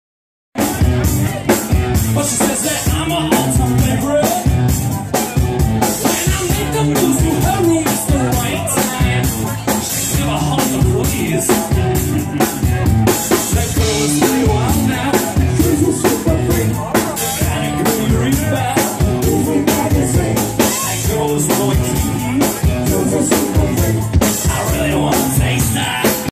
PG PUNCH BAND APRIL 2014(1)
Video of punch band performing at ducks beach club in North myrtle beach sc taken by sheila world of photography